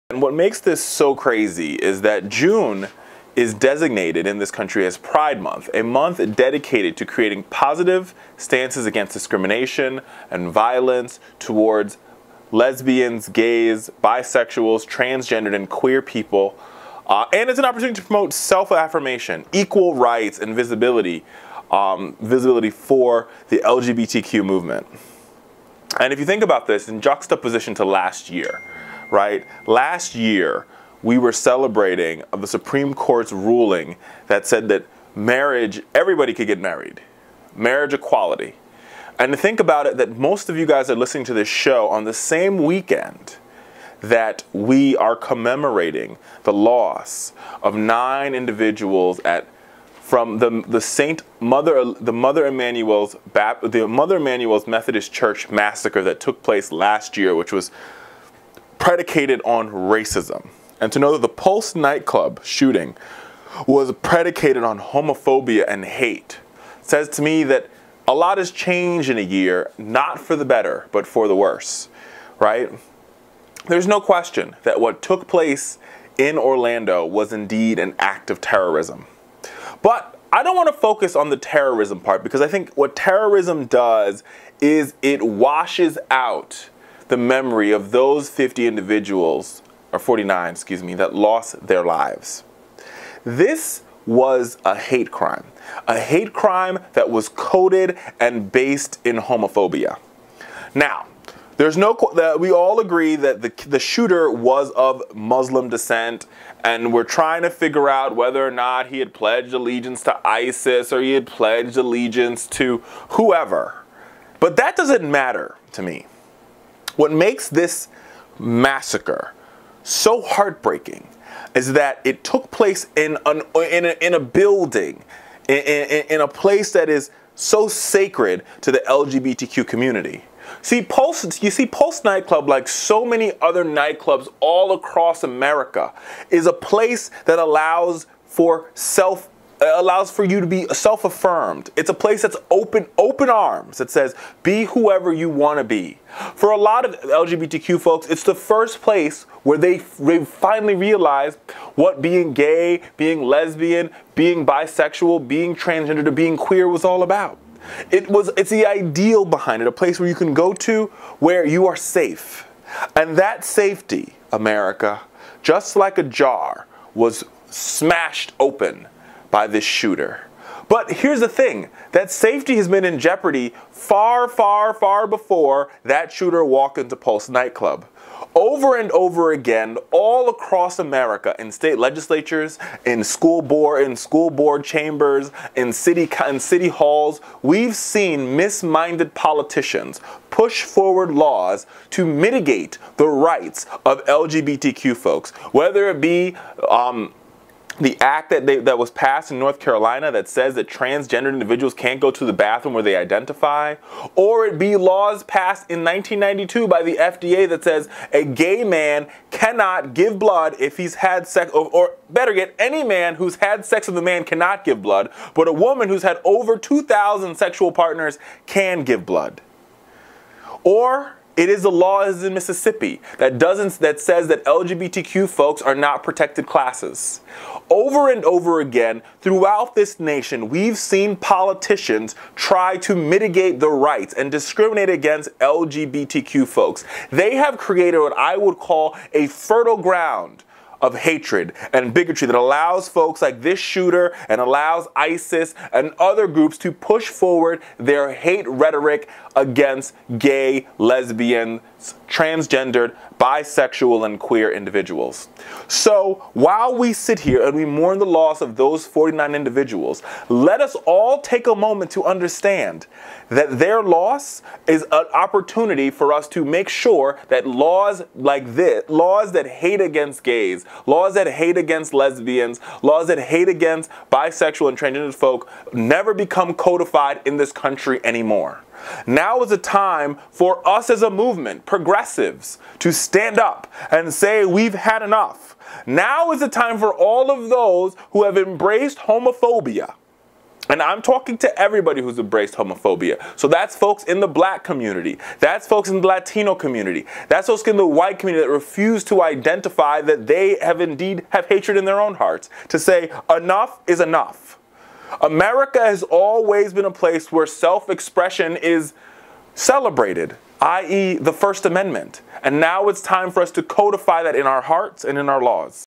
Orlando Attack Was A Hate Crime
June is Pride Month, a month dedicated to creating a positive stance against discrimination and violence toward lesbian, gay, bisexual, transgender and queer people and to promote their self-affirmation, equal rights, dignity and visibility as a social group. One year after celebrating the most joyous pride month in American history, coinciding with the Supreme Court ruling that legalized same-sex marriage, this year our nation is faced with mourning and sorrow. Many out there are angry, upset and in complete disbelief about what took place at the Pulse nightclub in Orlando, Fla.

Was it an act of terror? Yes. Was it a hate crime? Of course. Was it an act coated and based in homophobia? Absolutely. While some of our elected leaders and journalists have classified this incident as a hate crime against the LGBTQ community, others have made the cowardly choice of trying to erase LBGTQ folks from the tragic event. What happened almost a year ago in Charleston, S.C. at Mother Emmanuel Methodist Church was a hate crime against the African-American community. And if this weekend's incident had taken place in a Jewish synagogue or a Baptist church, it would have been immediately deemed a hate crime.

3 Steps To Join FowlerNation!
2. 'Like' The Richard Fowler Show on Facebook
3. 'Follow' Us on Twitter

Truthservers.com